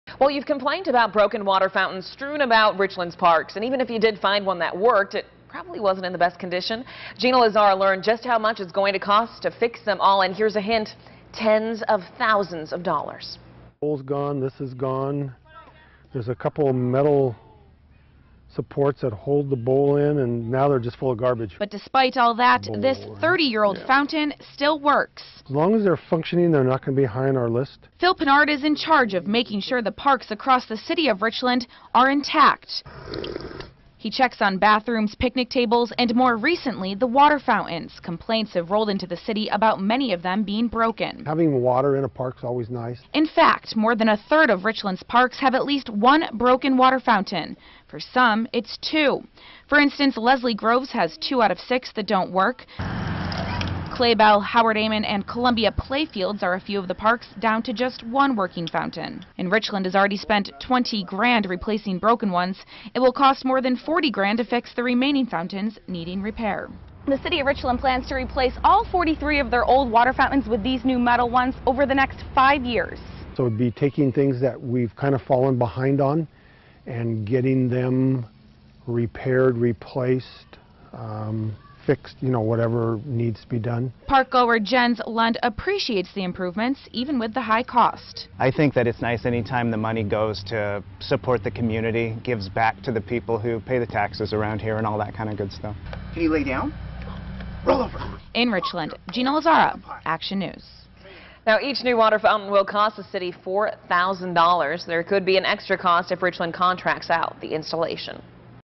Broken water fountains being fixed for tens of thousands in Richland
You've complained about broken water fountains strewn around Richland's parks. Even if you find one that works, it might not be in the best condition. We learned just how much it's going to cost to fix them all. Here's a hint: tens of thousands of dollars.